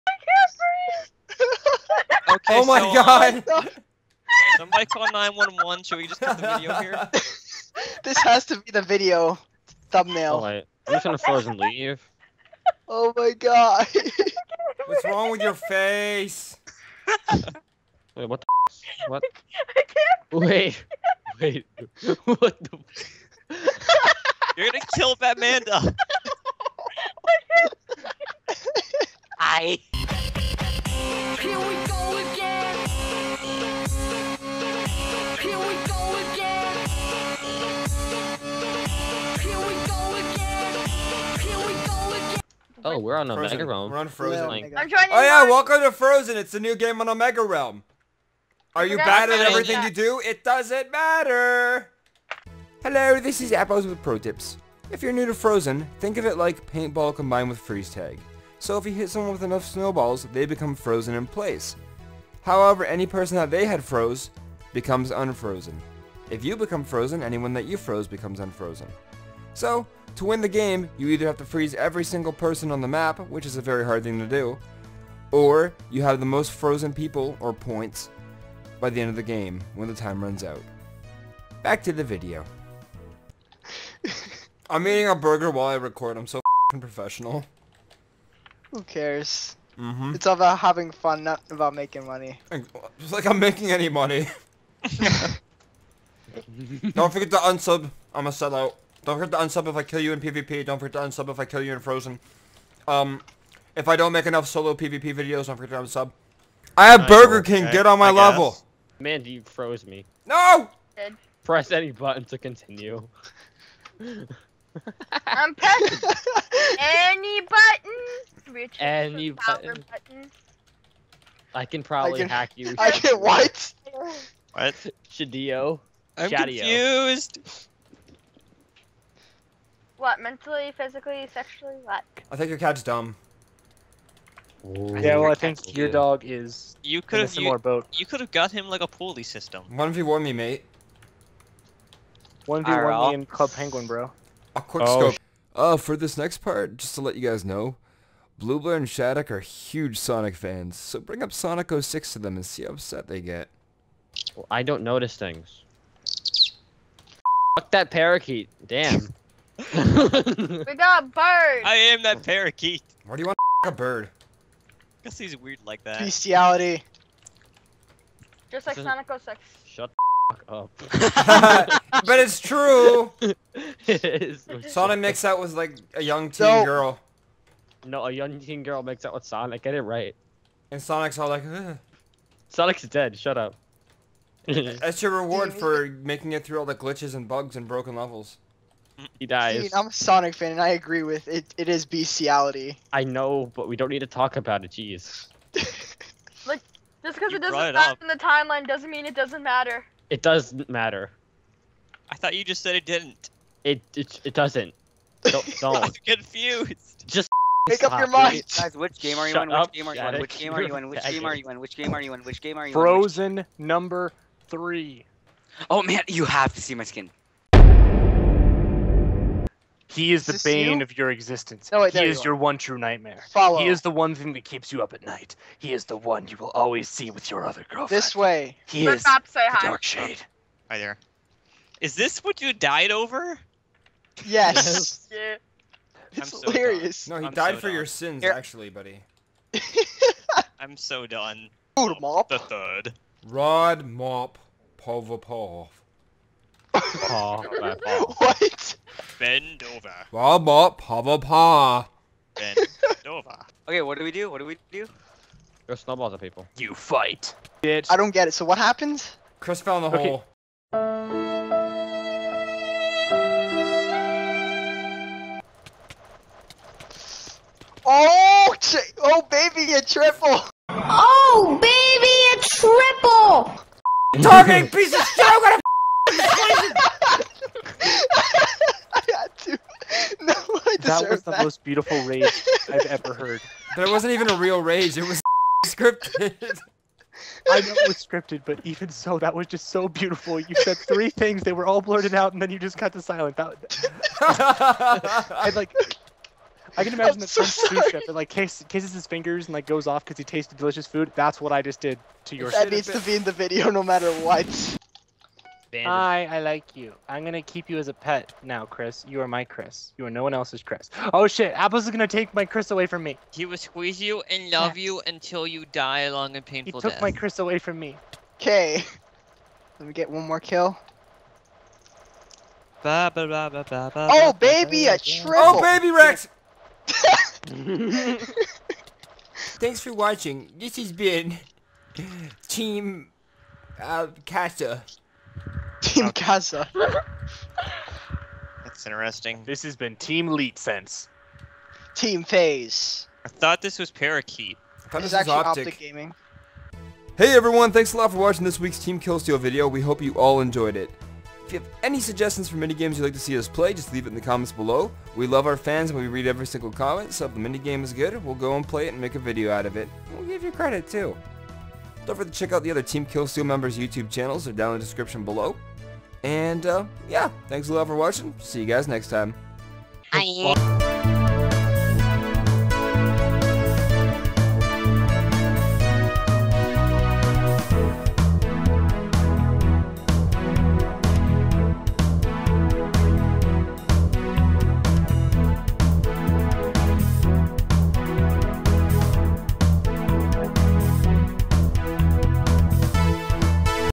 SELLOUTS - Team KIll Steal plays Frozen
TKS takes a look at Frozen, the new minigame on the Omega realm Server! Here are the best parts of the games we played!

♦♦♦♦♦♦♦♦♦♦♦♦♦♦♦♦♦♦♦♦♦♦♦Open Me♦♦♦♦♦♦♦♦♦♦♦♦♦♦♦♦♦♦♦♦♦♦♦

Resource Pack: Ozocraft custom edit by RedMapPVP